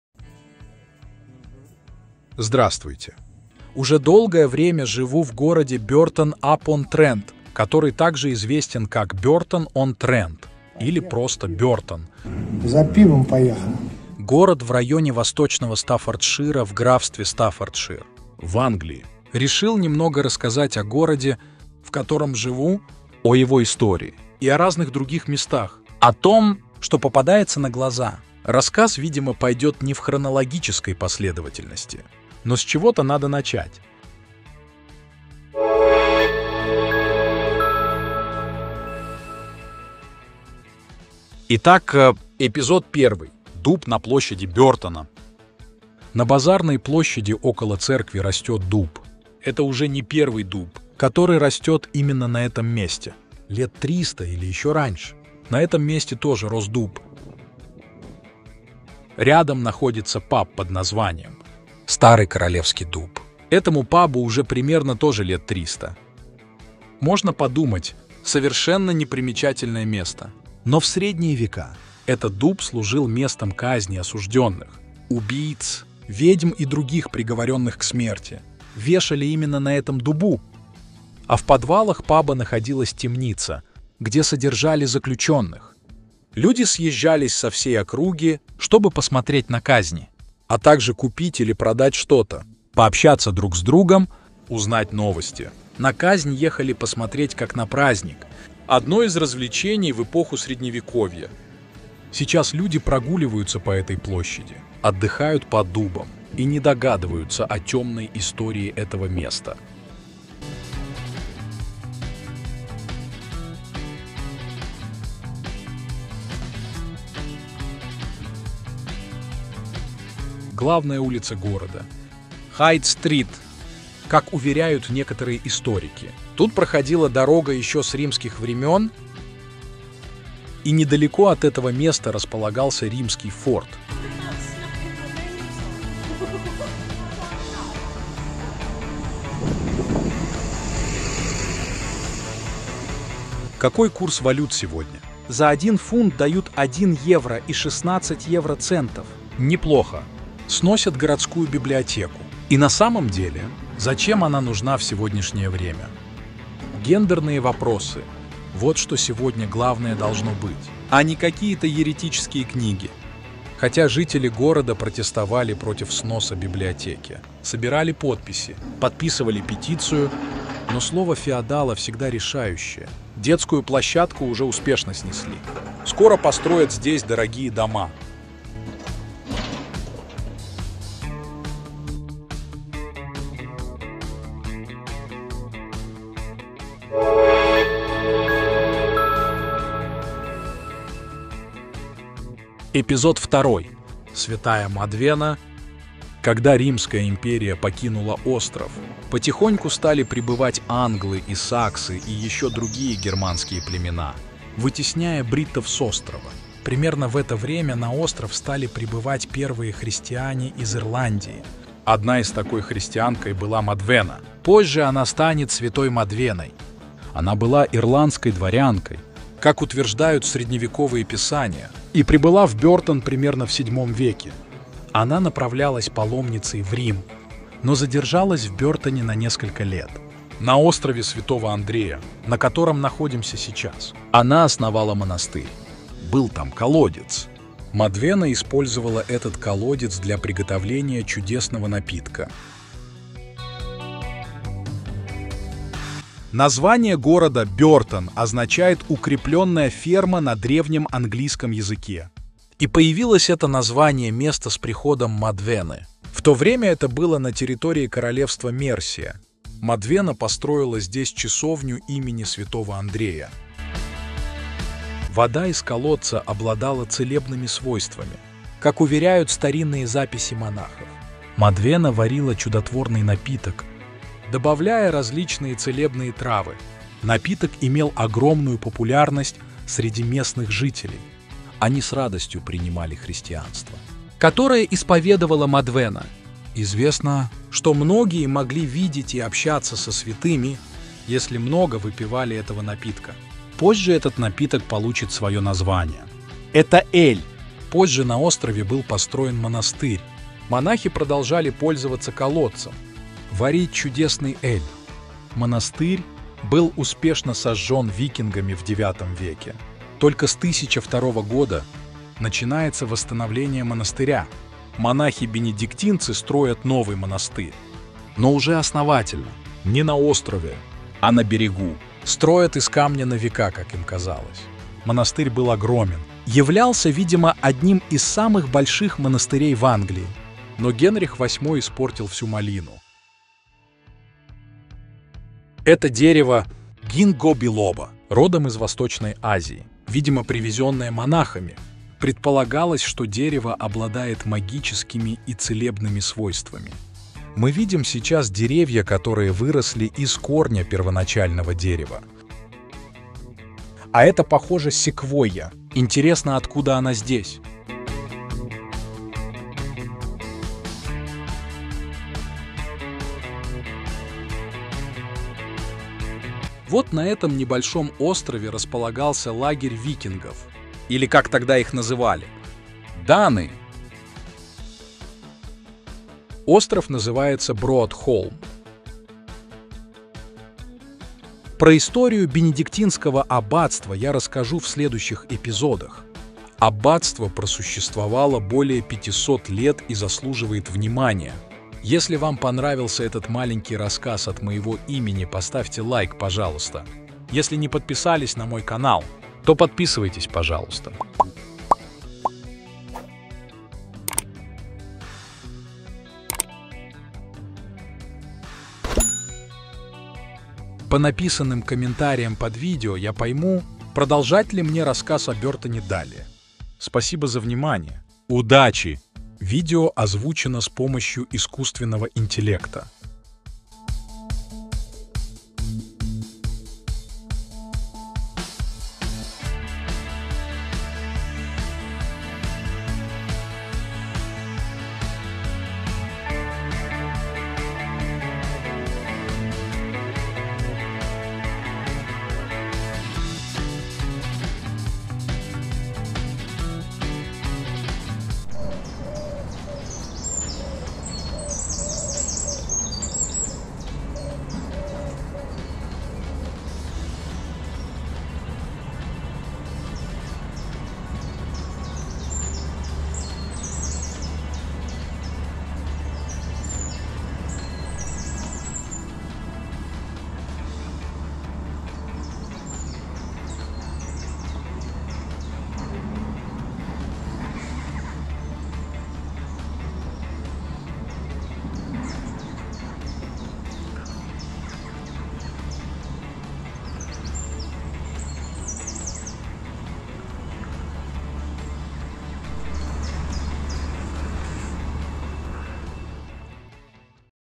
#1. Бёртон он Трент [ Burton upon Trent ] История. Англия.
#1. Бёртон он Трент [ Burton upon Trent ] История. Стаффордшир,Англия.
Эпизод 1. Дуб на площади. Старое место казни. Отрывки истории.
Эпизод 2. Святая Мадвена. Чудеса с элем.